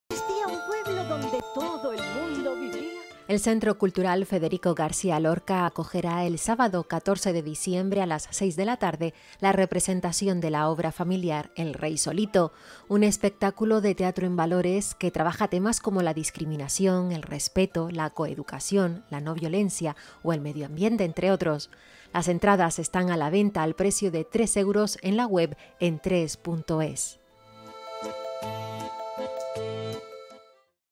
INGENIO ACOGE EL ESPECTÁCULO DE TEATRO EN VALORES “EL REY ZOLITO”
-Se representará el 14 de diciembre en el Teatro Federico García Lorca, estando las entradas a la venta en entrees.es-

El Centro Cultural Federico García Lorca acogerá el sábado, 14 de diciembre, a las 18 horas, la representación de la obra familiar “El Rey Zolito”, un espectáculo de teatro en valores que trabaja temas como la discriminación, el respeto, la coeducación, la no violencia, el medioambiente o la animación a la lectura entre otros. Las entradas están a la venta al precio de 3€ en la web entrees.es.

“El Rey Zolito” cuenta la historia de un rey que tenía un carácter peculiar por lo que estaba siempre discutiendo con las gentes de su reino. Su intransigencia e incomprensión provocaba muchísimas injusticias, pero a él todo le daba igual porque podía hacer lo que le daba la real gana, que para eso era el rey. El rey Zolito consigue que su castillo y su reino se conviertan en un mundo solitario y triste. Aunque nada importa, porque nuestro rey es capaz de inventarse todo un mundo imaginario donde todo existe y nada ha cambiado, aunque él sea el único habitante de su reino.